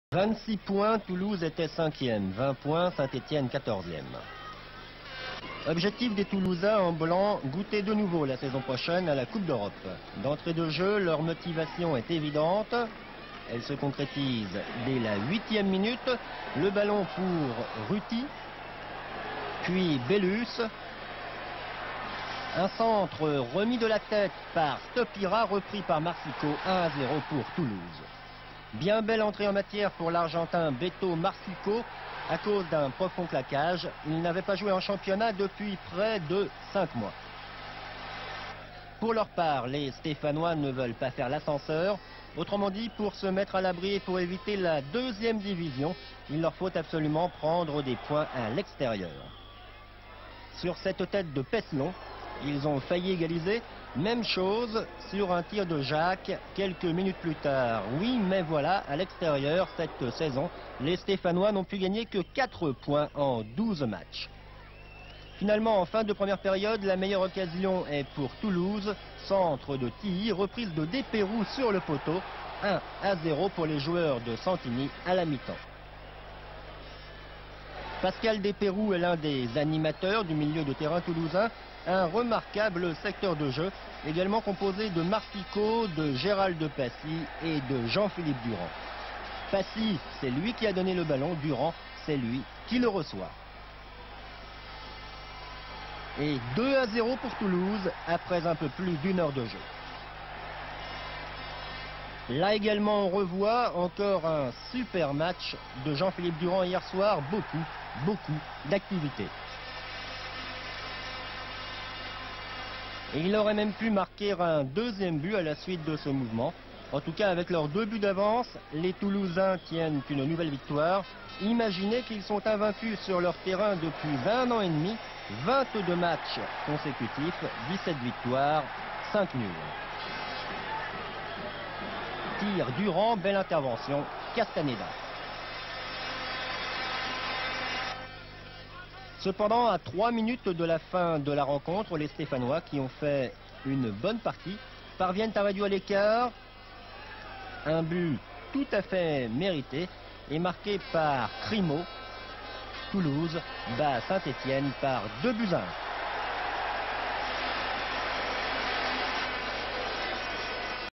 Toulouse 2-1 ASSE - 24e journée de D1 1986-1987
Après deux mois de trêve, cette petite mais nette défaite sur le terrain du Stadium face aux Toulousains des innombrables ex ou futurs Verts (Despeyroux, Passi, Lestage, Bellus, le coach Santini) menace les Verts de rélégation. Rappelons qu'ils viennent de remonter de deux ans de D2. Ils sont 15es.

Le buteur est Merry Krimau, le Marocain, qui sort d'un bon Mundial mexicain et sera le meilleur réalisateur stéphanois de la saison, avec ses quelques 9 buts. En fin de championnat, il inscrira notamment un doublé capital sur le terrain du Parc des Princes, face au Racing Paris, club qu'il rejoindra ensuite :

Pas manqué, il prendra les rênes de l'équipe à l'intersaison.

La saison suivante, sous sa direction, sera une réussite. Elle verra notamment une magnifique victoire dans la cité violette :

Images TF1